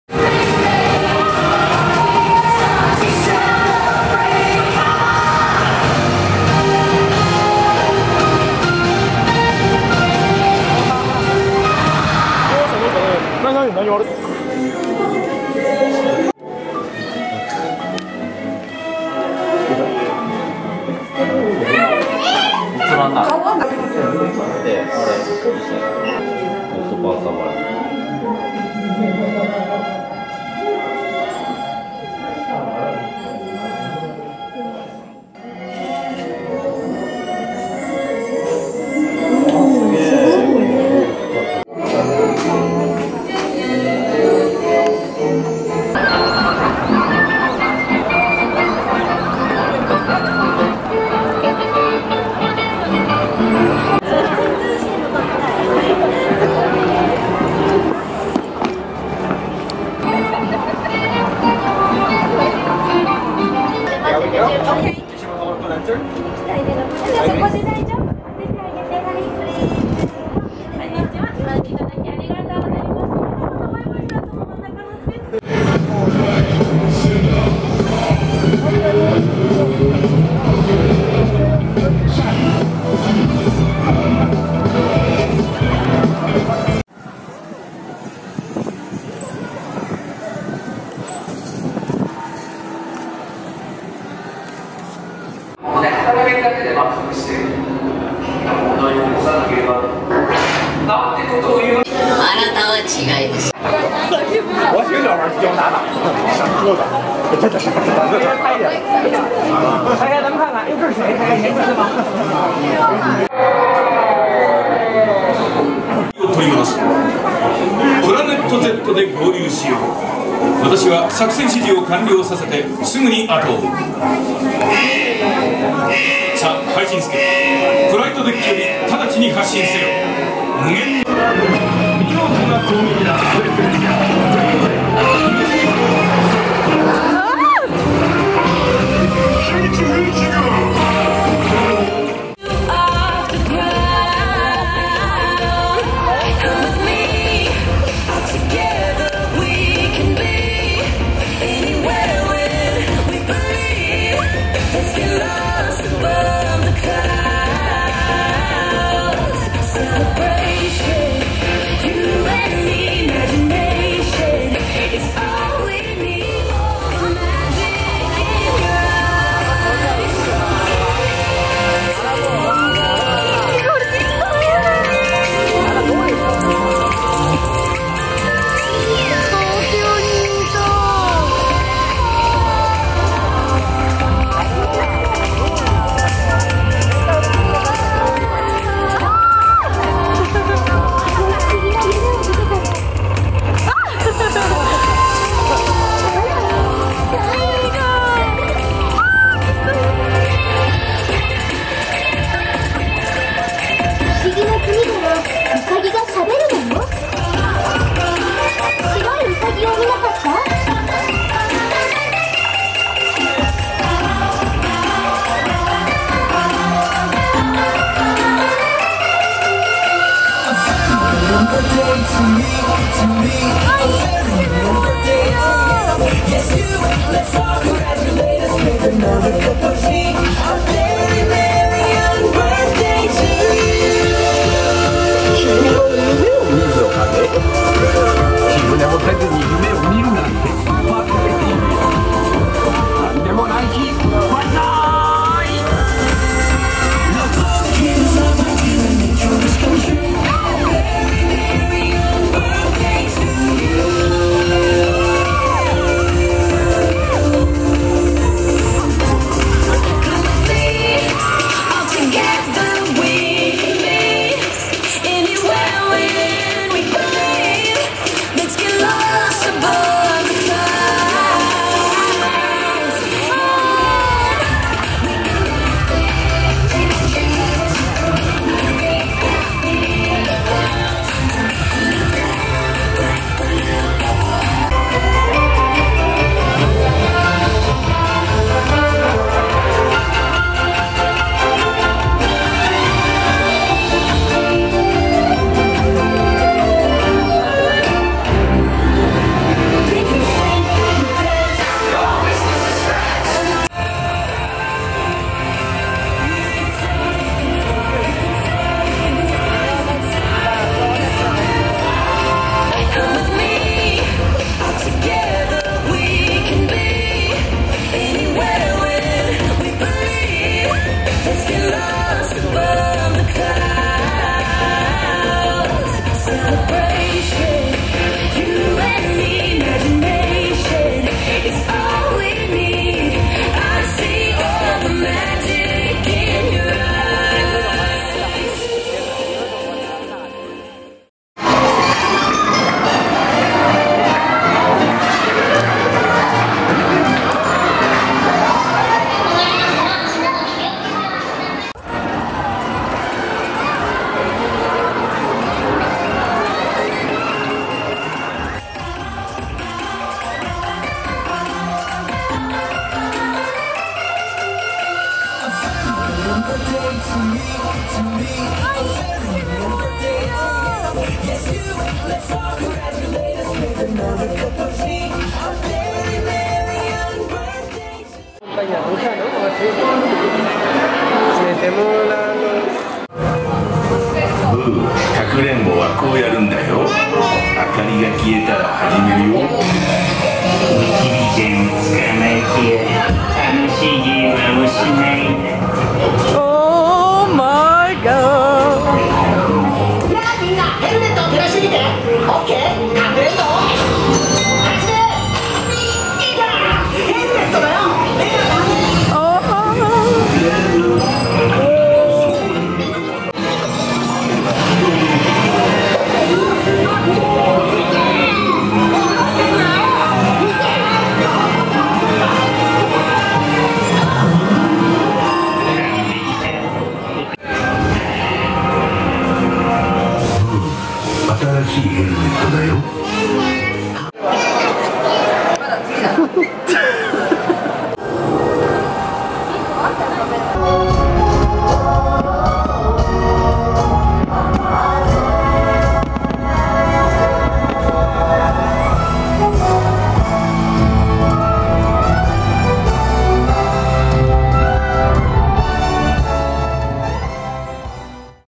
DISNEYLAND TOKYO ディズニーランド - Funkos con Noelia
Hoy os traigo mi vídeo de cuando estuve en DISNEYLAND , me encanta compartir estos momentos tan especiales con vosotros.

En ese enlace tenéis un descuento de POP IN A BOX!!